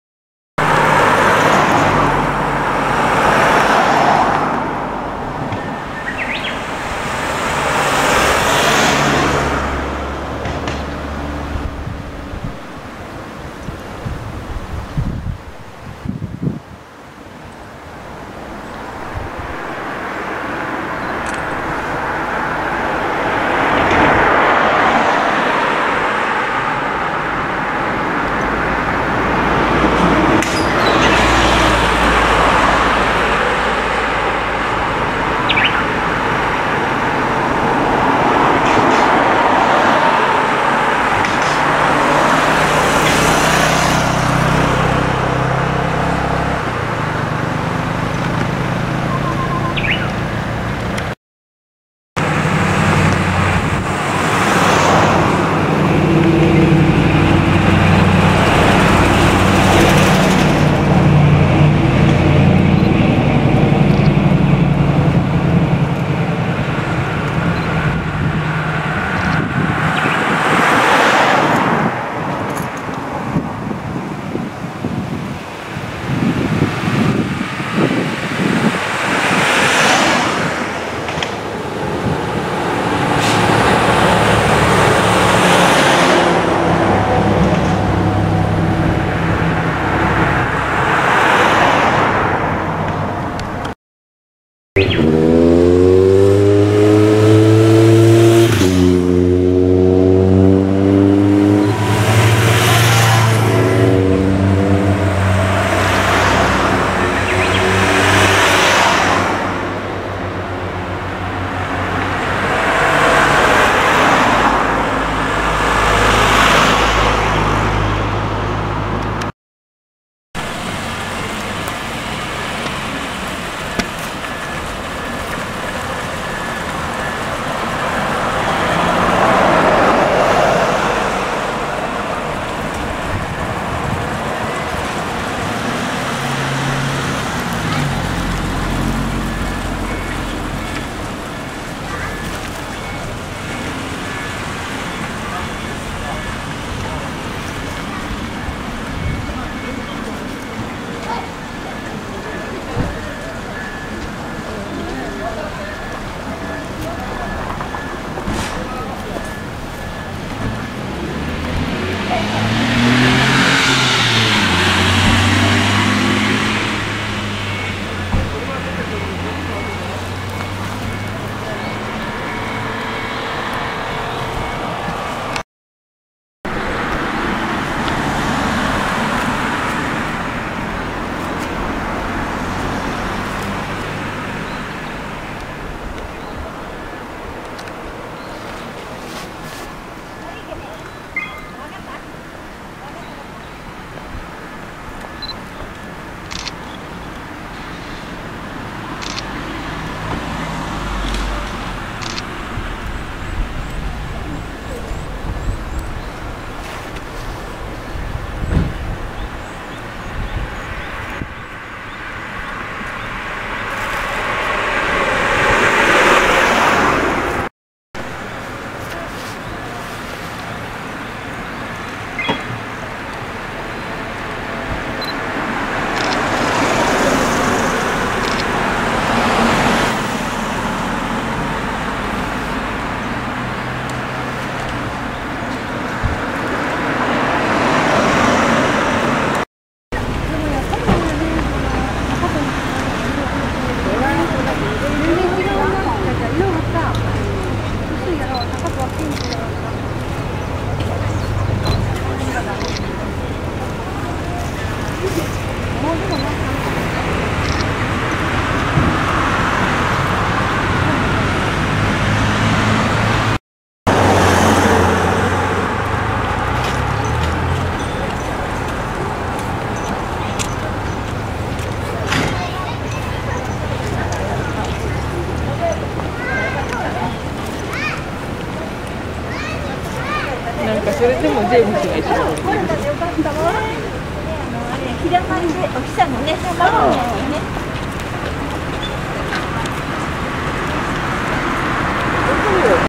Cherry Blossom Tunnel, Kawane Ieyama 31/Mar/2016 3川根家山の桜トンネル Túnel de Cerezo, Japón
Vehicles are passing under the cherry blossom. This is why this place is called Cherry Blossom Tunnel of Kawane Ieyama.
車は桜の花の下を通るので、この場所は桜トンネルと呼ばれています。
Los vehículos pasan por debajo de las flores de cerezo. Por lo tanto este lugar se llama el Túnel de Cerezo de Kawane Ieyama.
Kawane Ieyama, Shimada, Shizuoka, Japan 31/Mar/2016